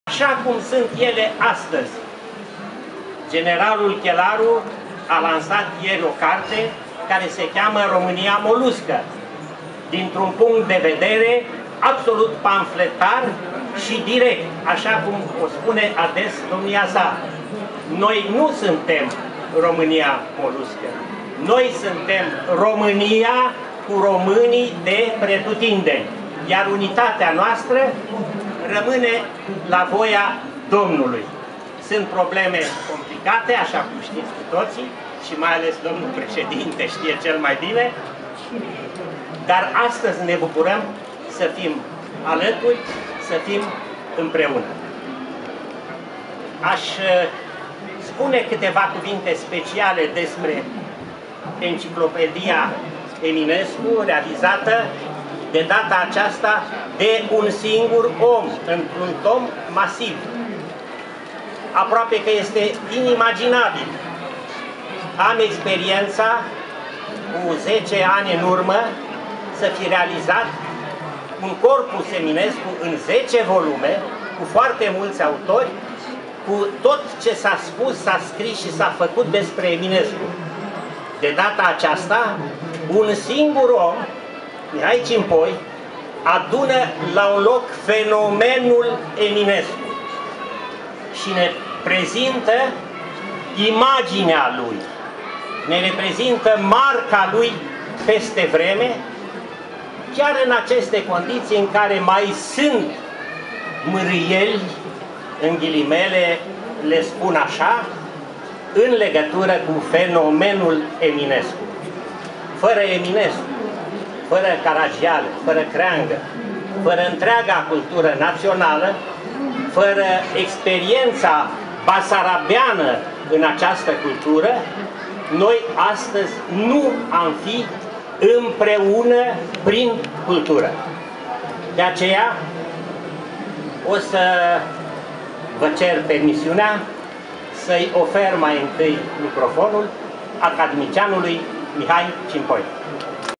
Victor CRACIUN - Lansare - Mihai EMINESCU - Mihai CIMPOI (Gaudeamus, 2012.11.24)
Victor CRĂCIUN vorbește la lansarea cărții: "Mihai EMINESCU. Dicționar enciclopedic", de academician Mihai CIMPOI.

Lansarea a avut loc la Târgul Internațional Gaudeamus - Carte de învătățură -, în 24 noiembrie 2012.

Dicţionarul enciclopedic Mihai EMINESCU, prima sinteză a eminescologiei mondiale, de acad. Mihai Cimpoi, a fost lansat la Târgul Internaţional Gaudemus.

În cele 1000 de pagini, lucrarea prezintă personalitatea, biografia, opera şi viaţa lui Eminescu, fiind analizate 340 de poezii eminesciene, prin prisma concepţiei lui acad. Mihai CIMPOI despre poezia modernă, existenţială.

Potrivit autorului, dicţionarul include şi noi reprezentări ale operei geniului în filatelie, medalistică, muzică, arta plastică din mai multe ţări.